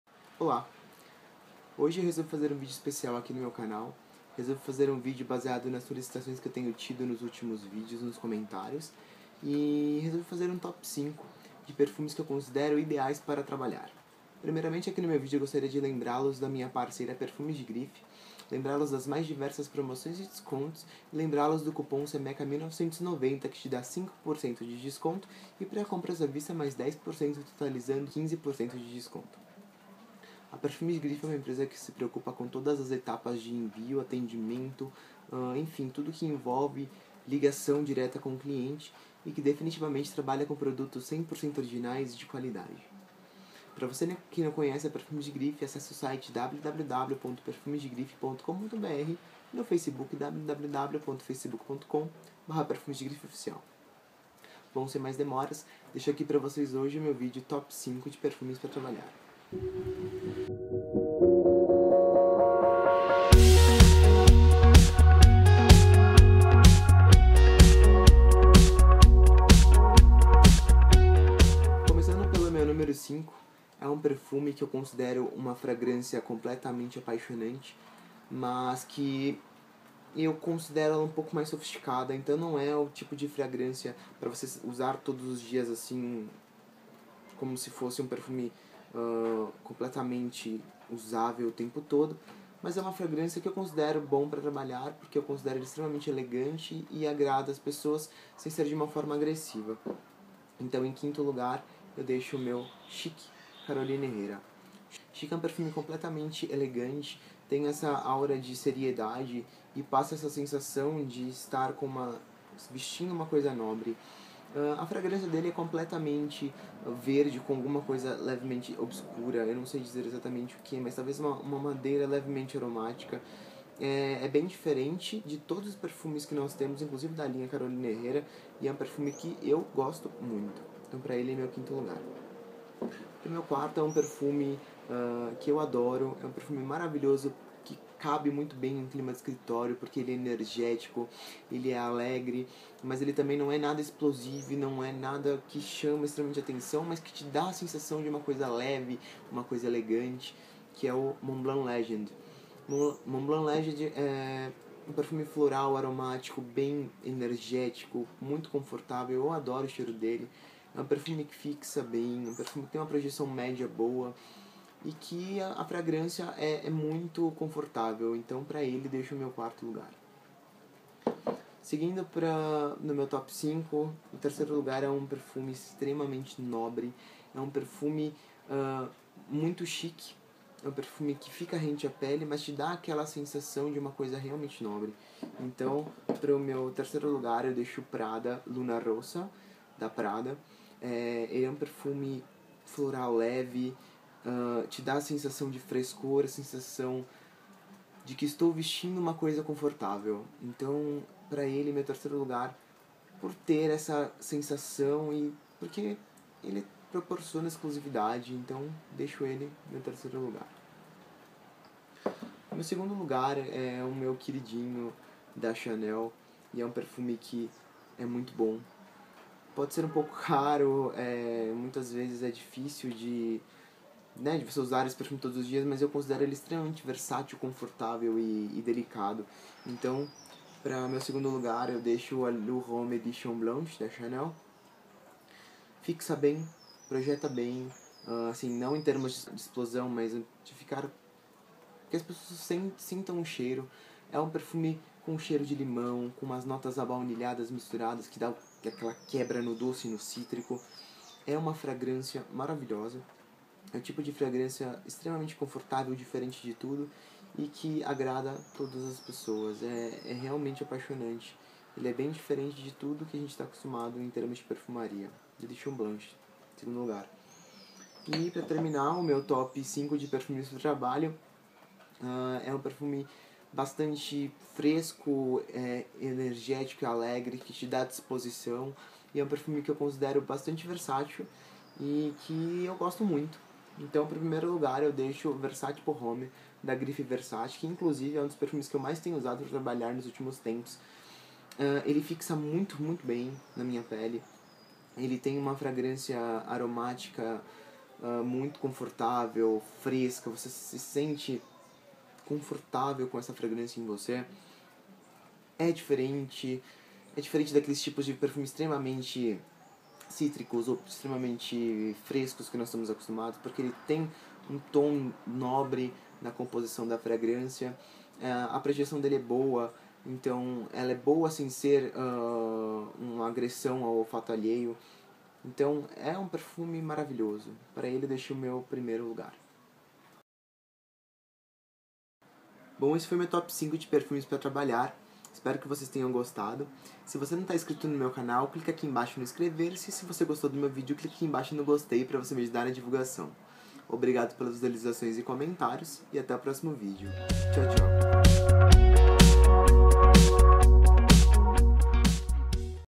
Especial Top 5 - Perfumes para Trabalho
Olá!

Devido às mais diversas solicitações que tive, resolvi fazer um vídeo especial contando quais perfumes (no meu gosto pessoal) mais eu acho agradáveis para trabalhar.

Espero que gostem, e que compartilhem das suas opiniões com a gente!

Grande abraço!